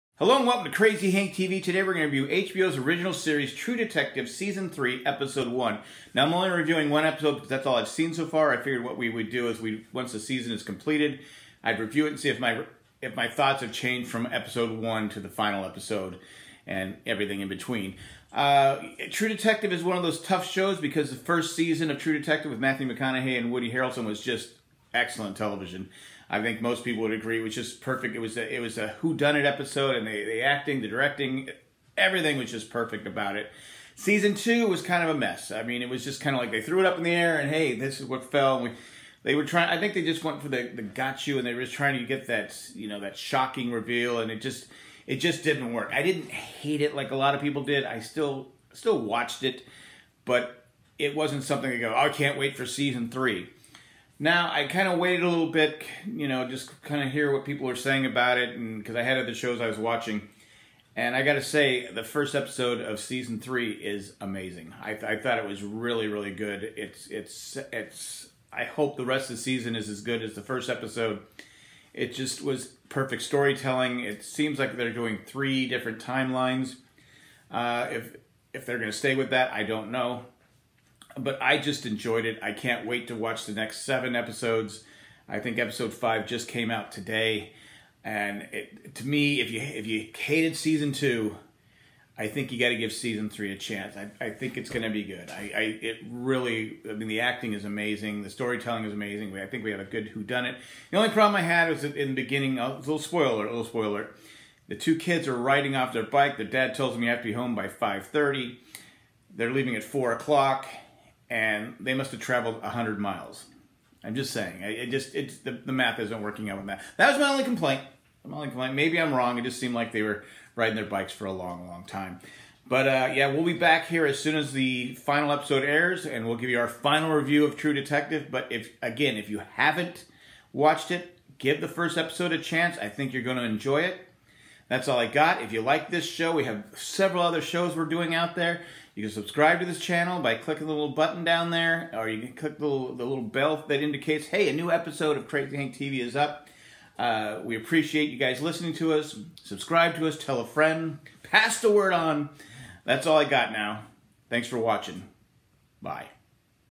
True Detective season 3, The Great War and Modern Memory
We review season 3 episode one of HBO's original series of True Detective. Starring Mahershala Ali, Carmen Ejogo, Stephen Dorff, Scoot McNairy, Ray Fisher, Jon Tenney, Josh Hopkins, Mamie Gummer, Sarah Gadon and Rhys Wakefield.